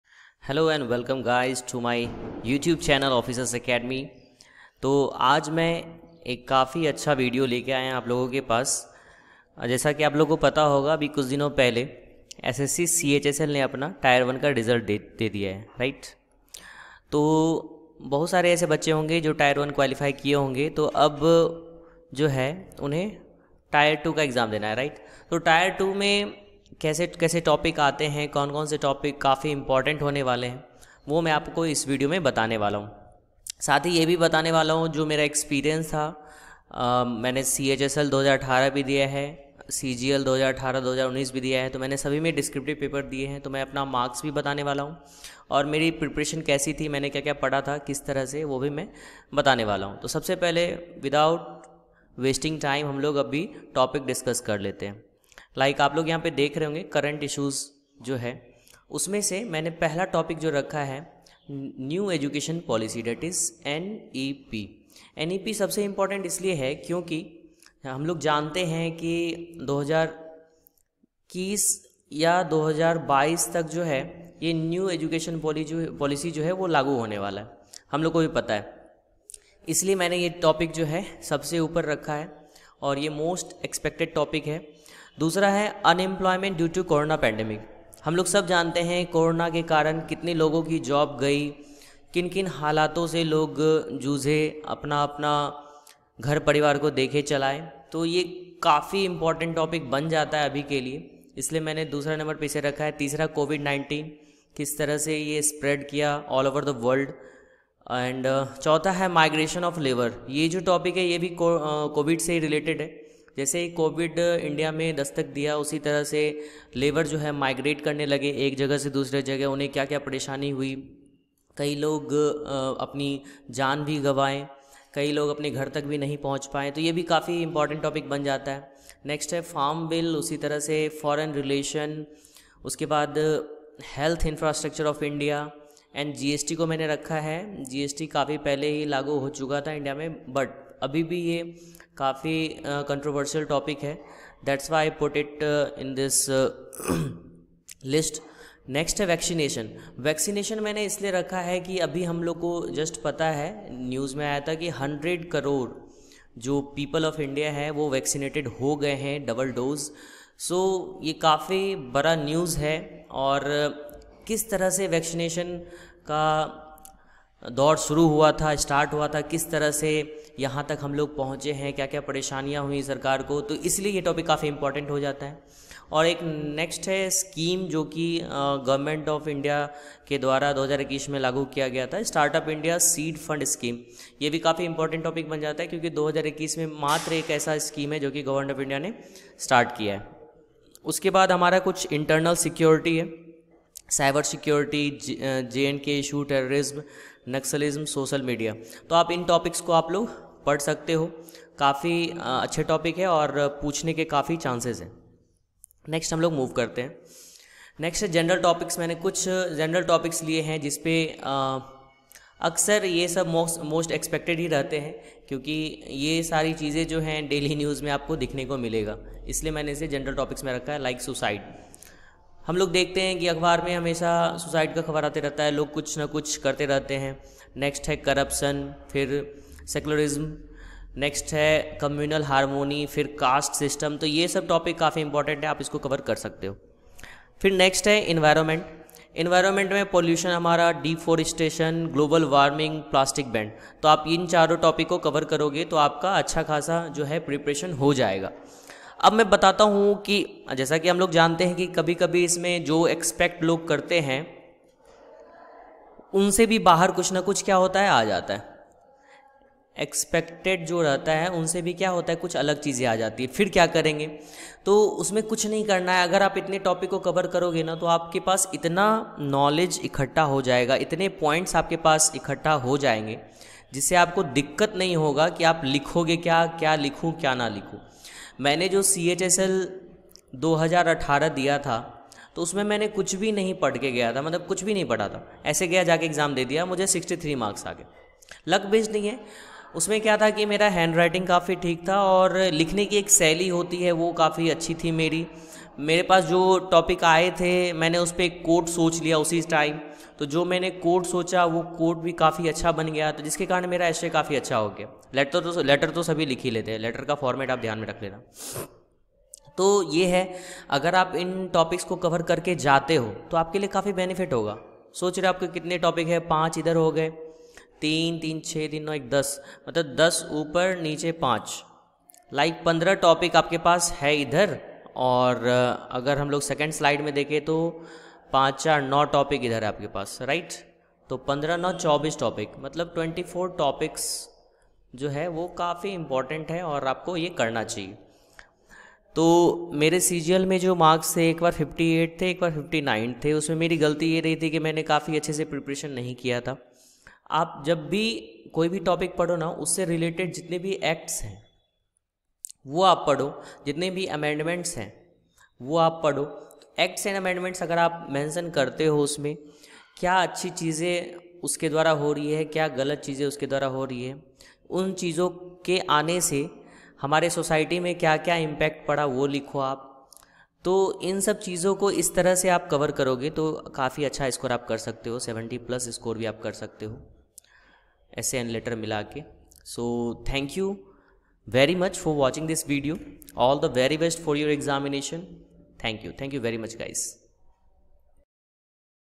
SSC CHSL 2020 Tier 2 || Most Expected Essay Topic || SSC CGL 2020 Tier 3 or Descriptive Paper
In this video i have suggested some most relevent topic for the ssc chsl tier 2 and ssc cgl 2020 tier 3.

SSC CHSL 2020 Tier 2 || Most Expected Essay Topic || SSC CGL 2020 Tier 3 or Descriptive PaperTelegram